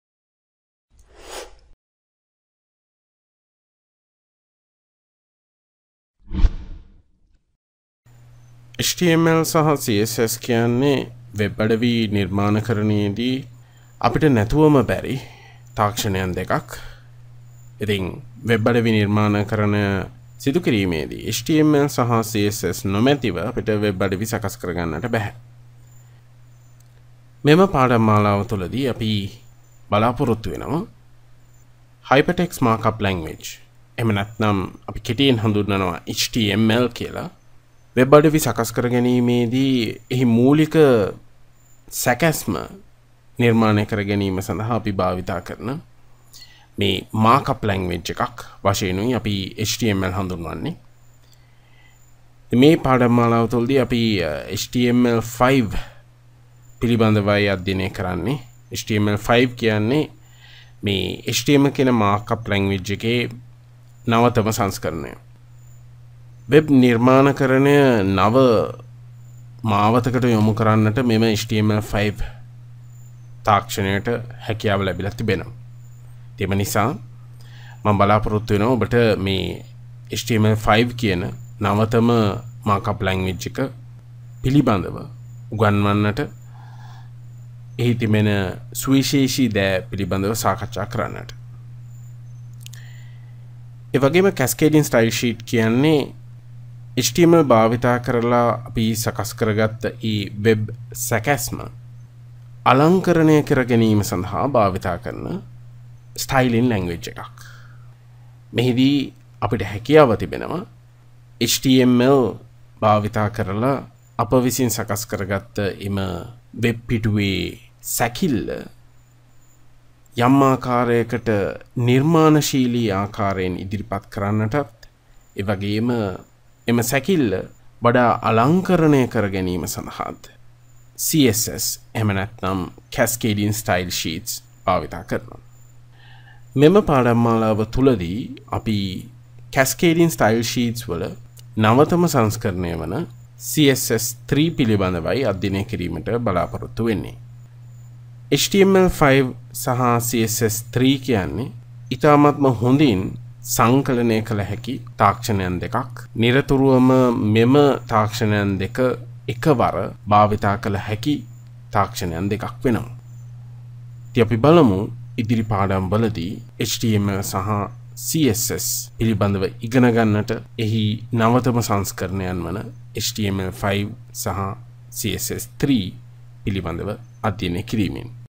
Introduction to the HTML5 and CSS3 Course | වෙබ් පිටු නිර්මාණය කරන ආකාරය මුල සිට ඉගෙන ගමු
Learn HTML5 and CSS3 in Sinhala. Hypertext Markup Language 5 සහ Cascading Style Sheets 3 යන තාක්ෂණයන් දෙක වෙබ් පිටු නිර්මාණය කිරීමේදී අනිවාර්‍යයෙන් දැන ගත යුතු විෂයන් දෙකක් ලෙස සැලකේ. අප ඉගෙන ගැනීමට බලාපොරොත්තු වන පාඩම්මාලාව පිළිබඳ සරල හැදින්වීමක්.

අමතර පාඩම් සහ තොරතුරු සඳහා අපගේ වෙබ් අඩවි වලට පිවිසෙන්න.